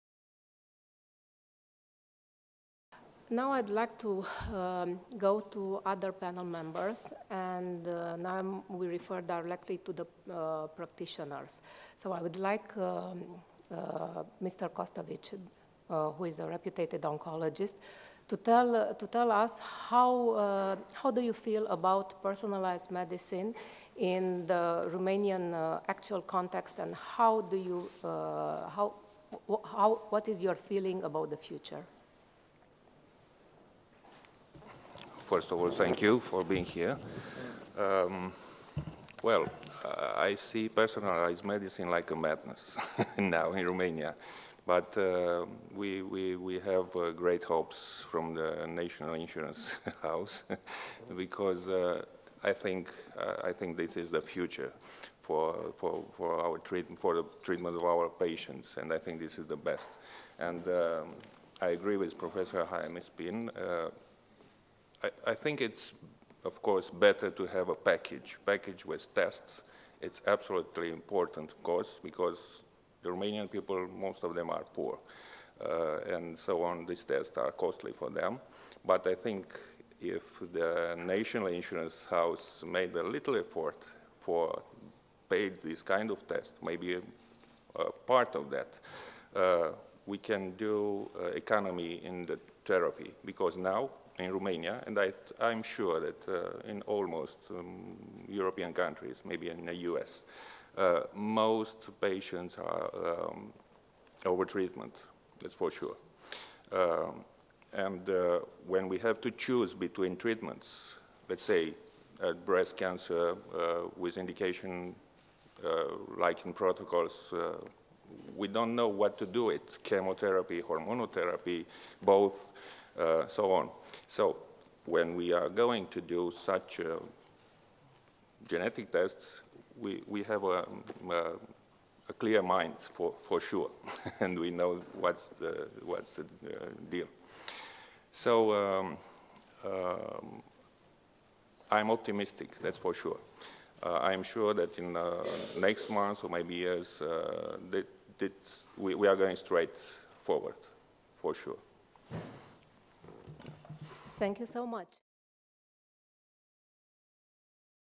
Cătălin Costovici at Bucharest Forum Healthcare 2015
Cătălin Costovici, MD, Diagnosis, Ambulatory Treatment and Preventive Medicine Medical Center, Romanian Academy, on “Personalized Medicine – The Long Road ahead for Medical Research and Development” panel, at Bucharest Forum Healthcare 2015, held at the National Institute for Statistics, on March 27.
Dr. Costovici started his medical carrer after graduating from the University of Medicine and Pharmacy of Craiova with a diploma of GMP and specialized in Medical Oncology while doing his residency at the Bucharest Institute of Oncology “Alexandru Trestioreanu”.
Focusing on cancer cases, therapy and treatment, he then followed a series of postgraduate courses, related to his field of study in Vienna, Malta, Rome, Edinburgh, Indianapolis, Bucharest and Hungary, while also publishing various articles in specialized journals and books.
At the same time, Dr. Costovici worked as a specialist in Medical Oncology at the Institute of Oncology in Bucharest, followed by various medical centers. He is also a member of ASCO (American Society of Clinical Oncology), ESMO (European Society of Clinical Oncology), IASLC (International Association for the Study of Lung Cancer), ESGO (European Society of Gynecologic Oncology).